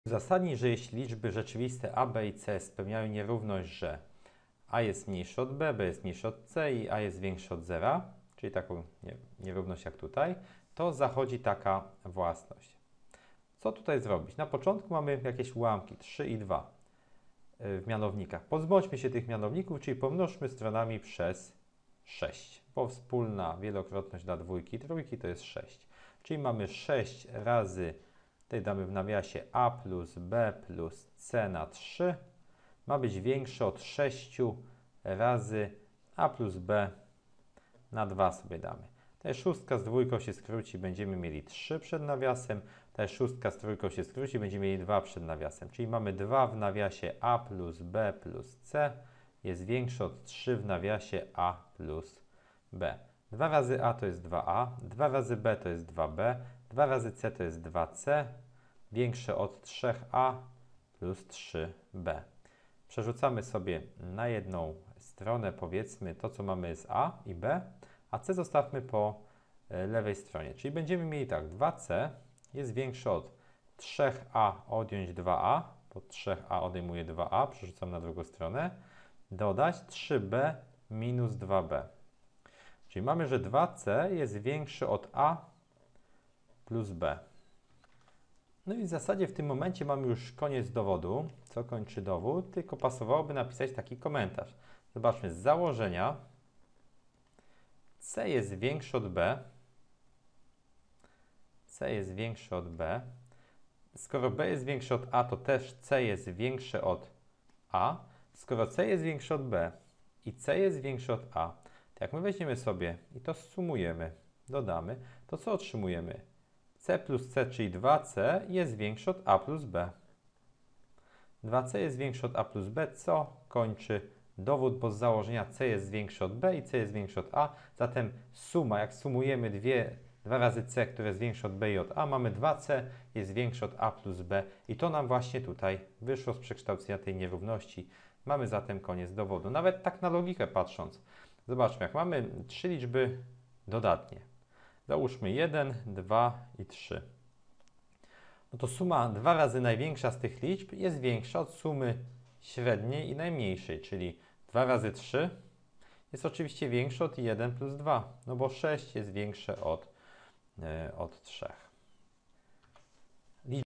27 Uzasadnij, że jeśli liczby rzeczywiste a,b,c spełniają nierówności 0abc, to a+b+c3a+b2
Uzasadnij, że jeśli liczby rzeczywiste a,b,c spełniają nierówności 0abc, to a+b+c3a+b2.